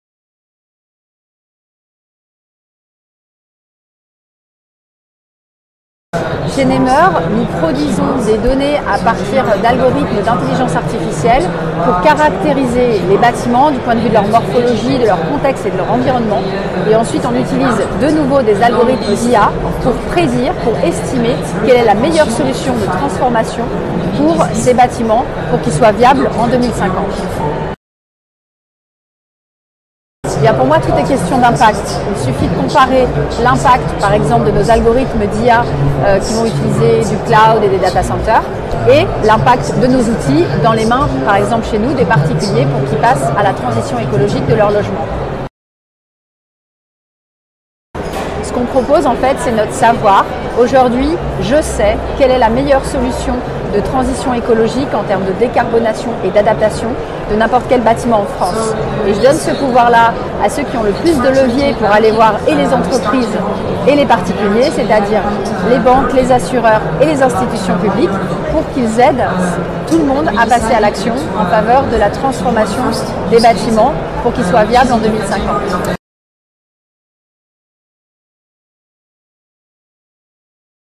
Chloe Clair at VivaTech 2024
Chloé Clair, CEO and co-founder of namR, presents the uses of Artificial Intelligence within namR in 1 minute.